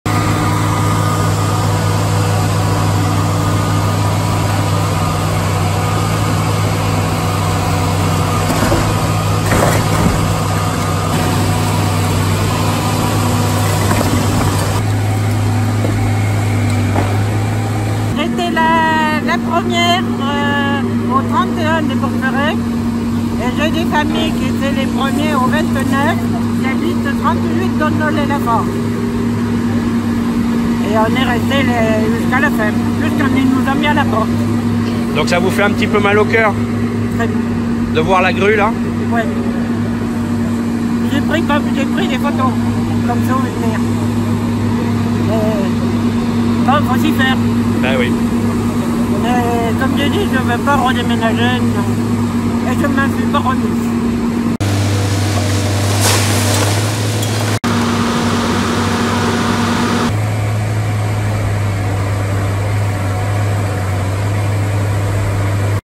Lancement du chantier de l'ecoquartier des Portes-Ferrees a Limoges
Les pelleteuses entament la destruction des anciennes barres d'immeubles du quartier des Portes-Ferrees, à Limoges.